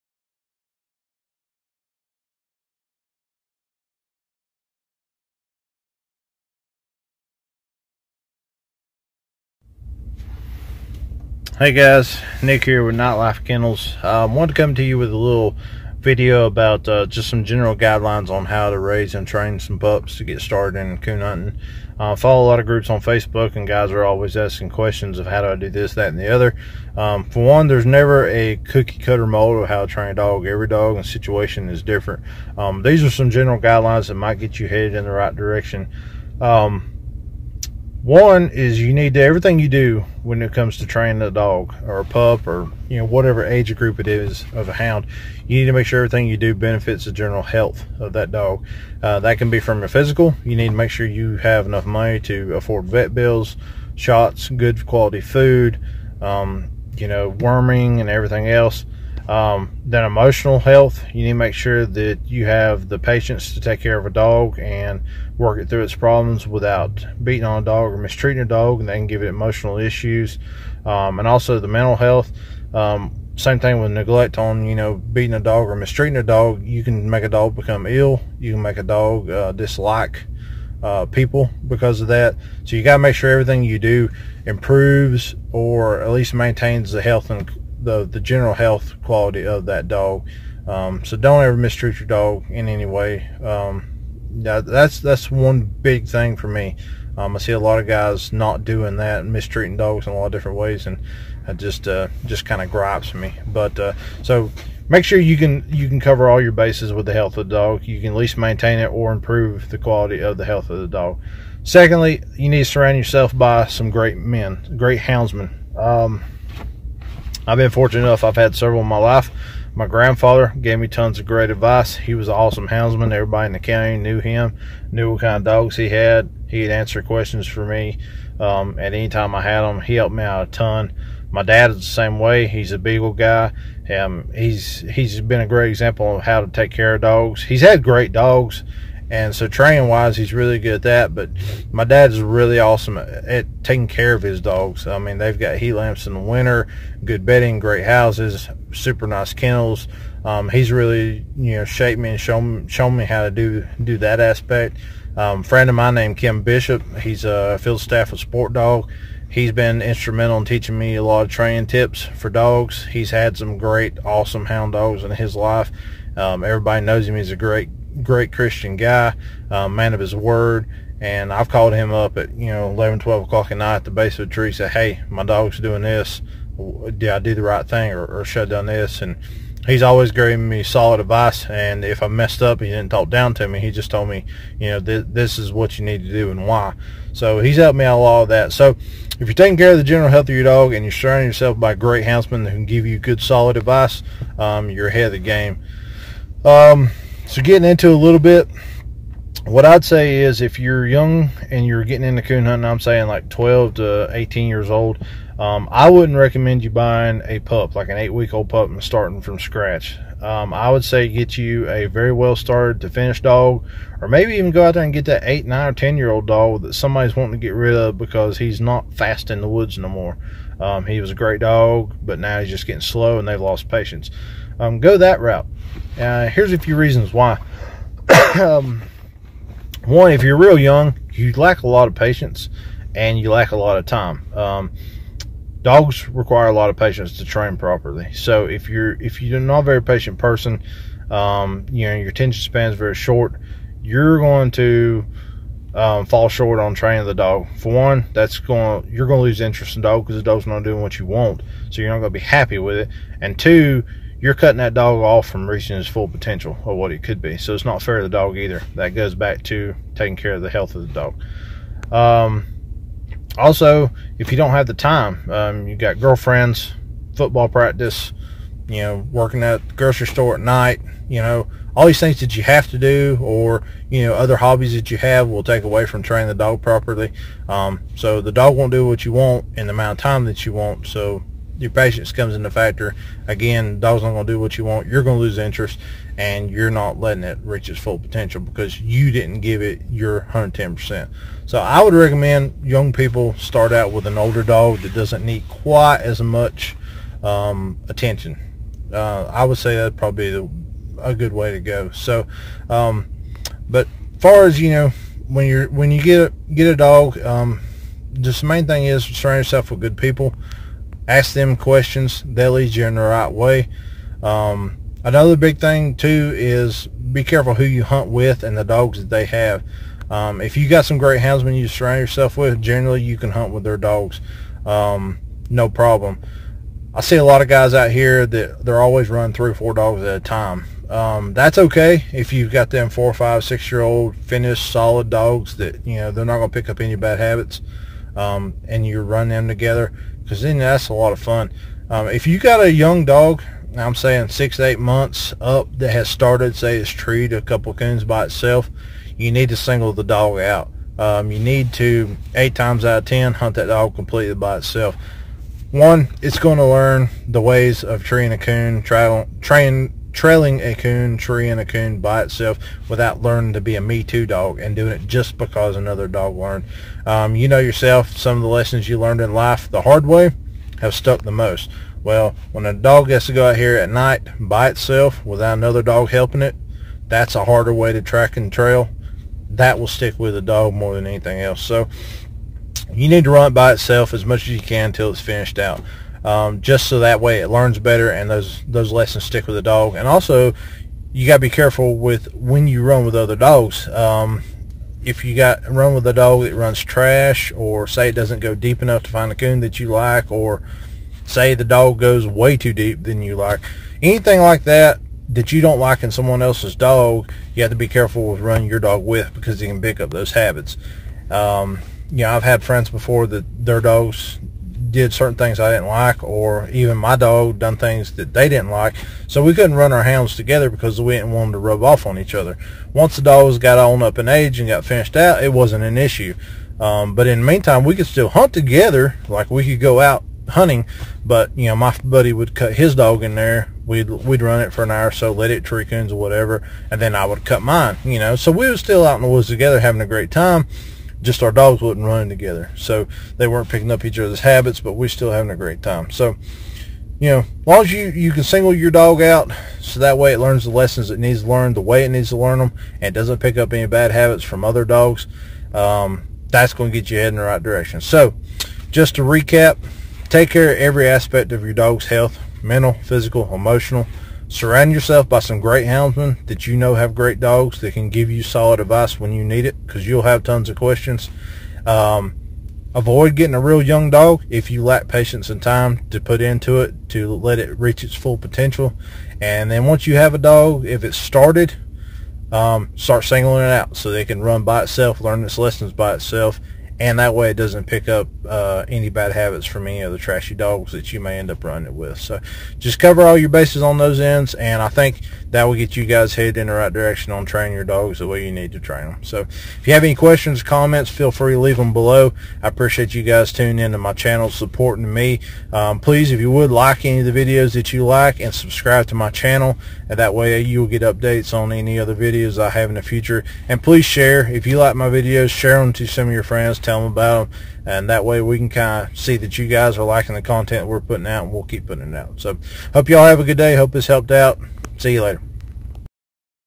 COON HOUND TRAINING ADVICE
Here's a few general guidelines to help new coon hunters get headed the right direction when training coon hounds!